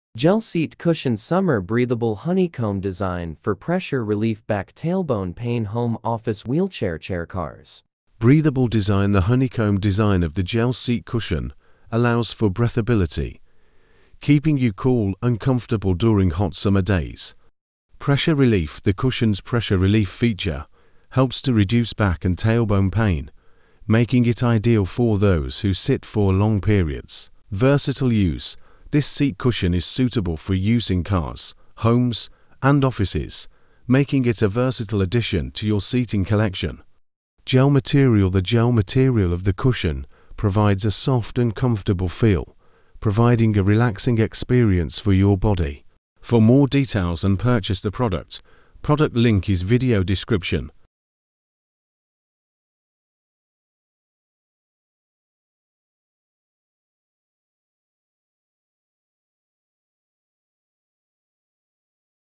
Beat Back Pain! Summer Gel Seat Cushion (Honeycomb Design)
Say goodbye to back pain with this amazing Gel Seat Cushion!   The honeycomb design keeps you cool in summer while the gel core provides pressure relief for your tailbone and back. Perfect for office chairs, wheelchairs, cars, and more!
product Link 🖇️🖇️🖇️